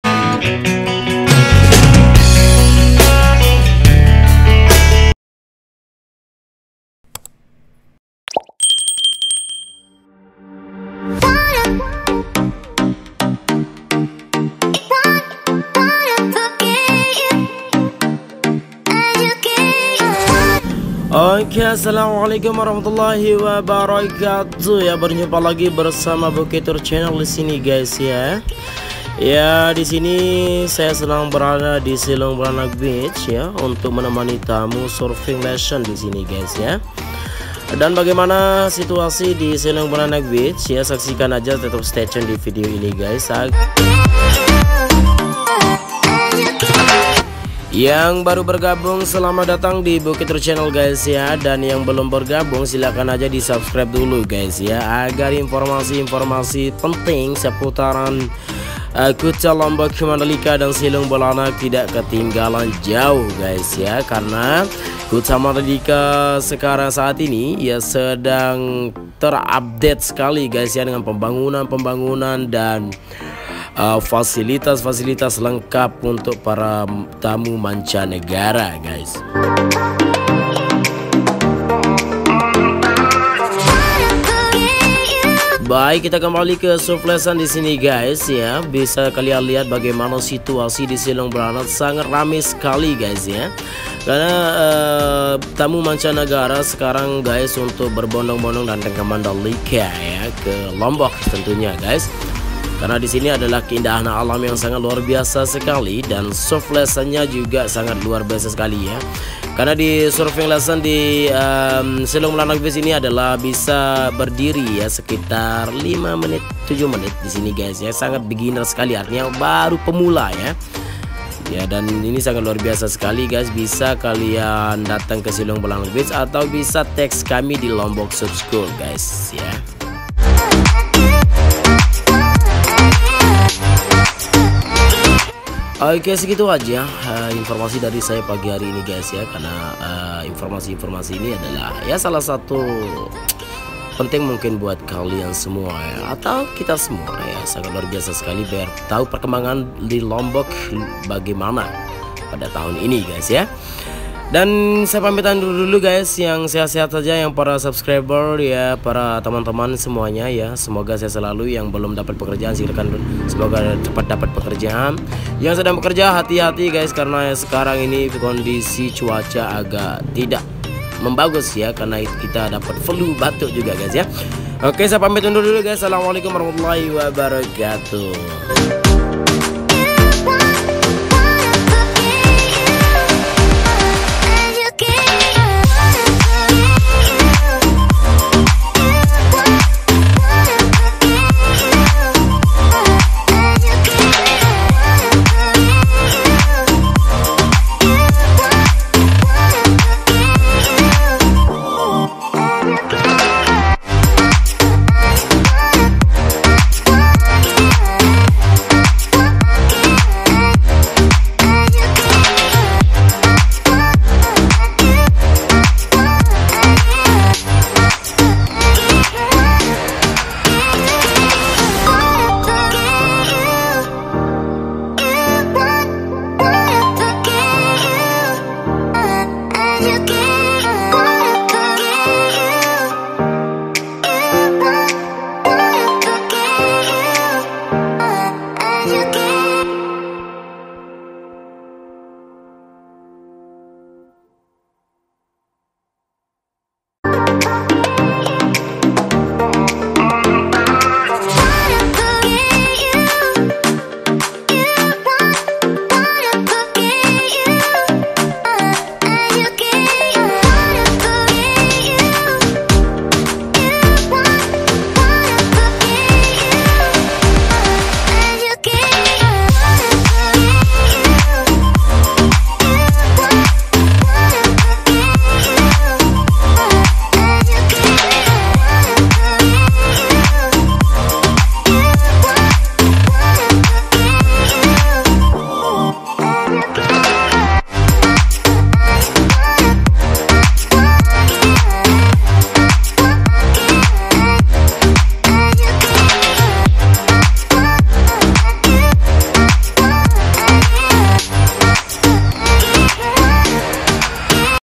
Situasi Di area selong blanak surfing lesson
selong blanak beach tempat wisatawan berselancar atau surf lesson termasuk area kuta mandalika